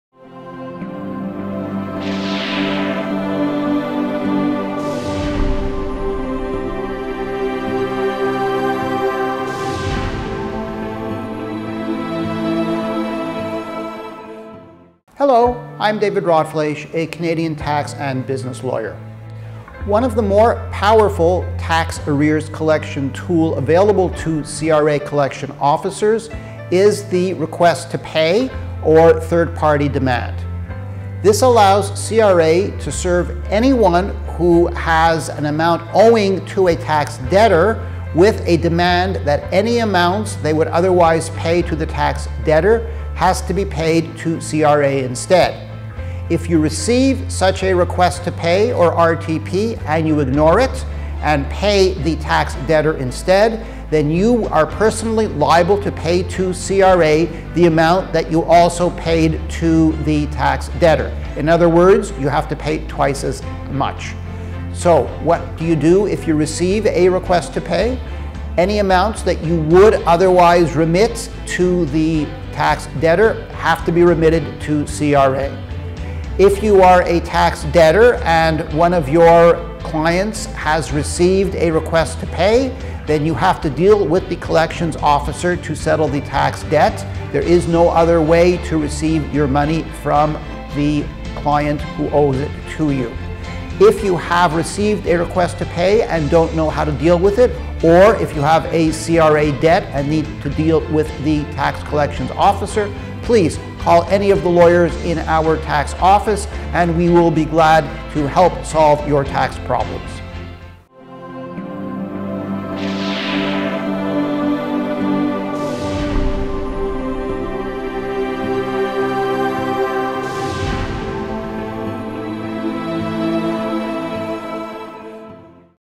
RTP
CRA can serve a request to pay (RTP or third party demand) on anyone who owes money to a tax debtor and the payments then have to be made directly to CRA.